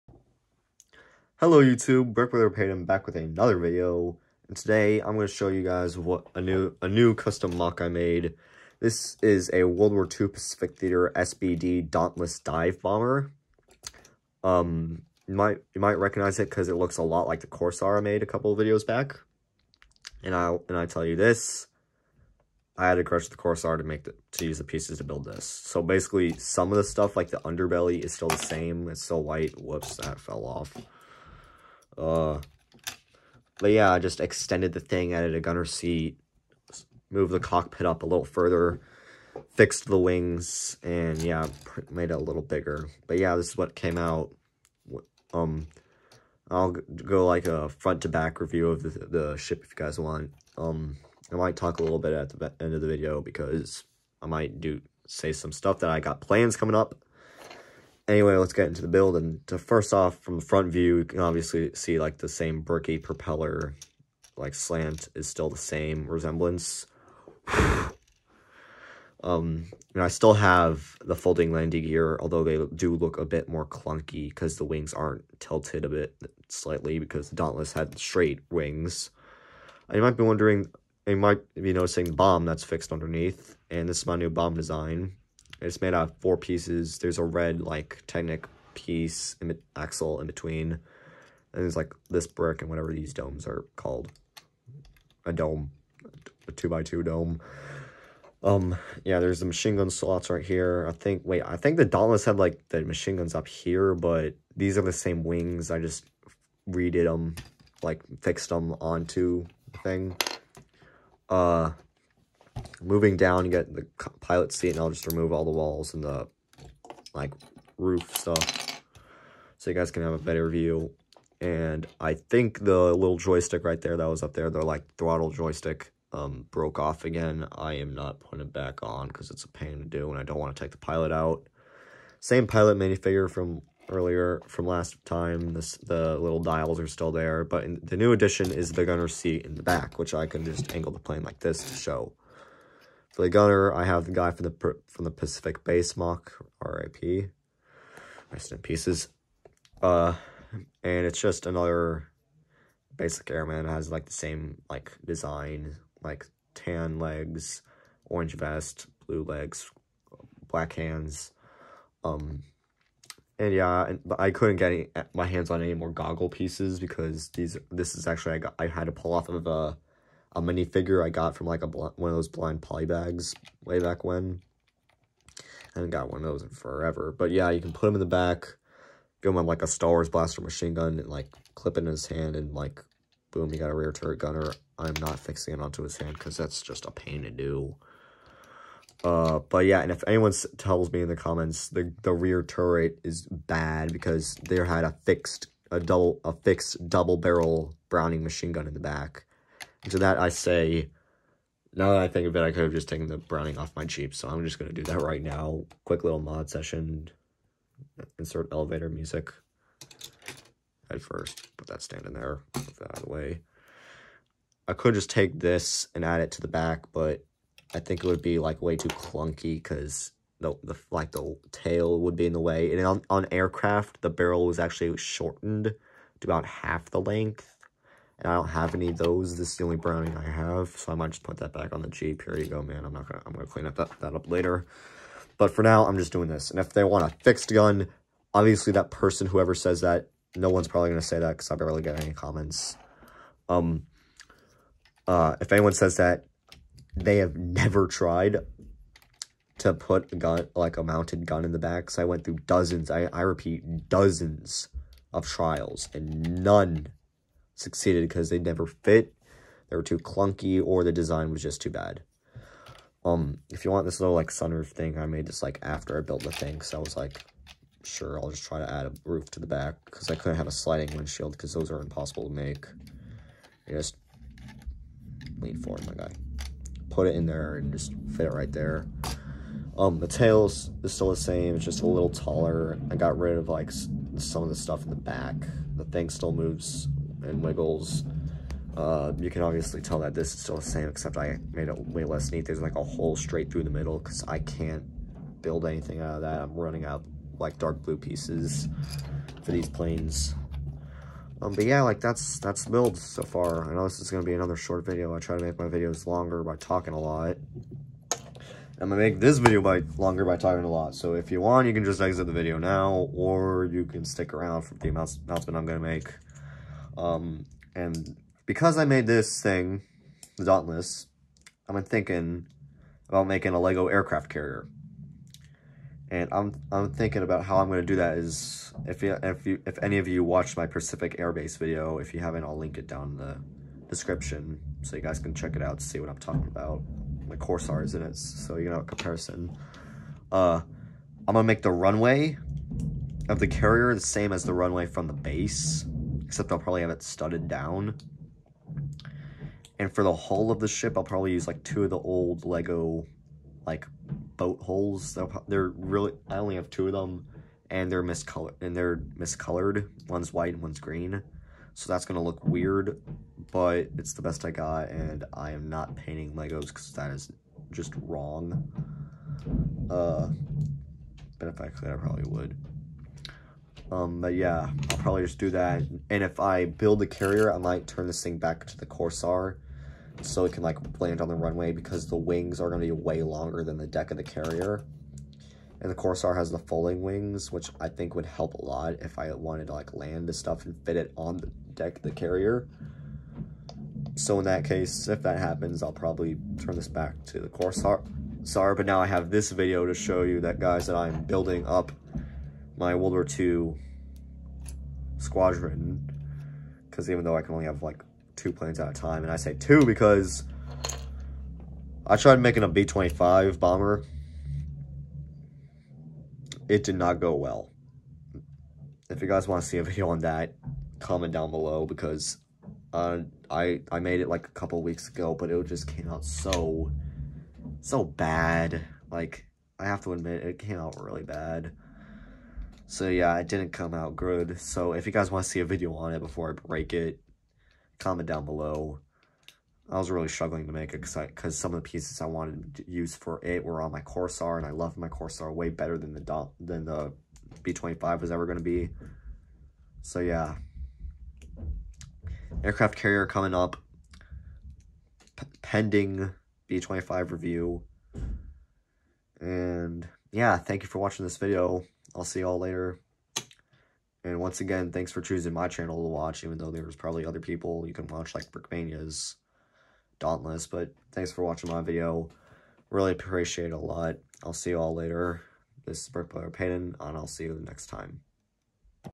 Lego World War II SBD Dauntless Dive bomber Moc
SBD Dauntless dive bonnet MOC converted from my Corsair fighter.
Comment if you want to see a aircraft carrier in Lego or if you like the Corsair better.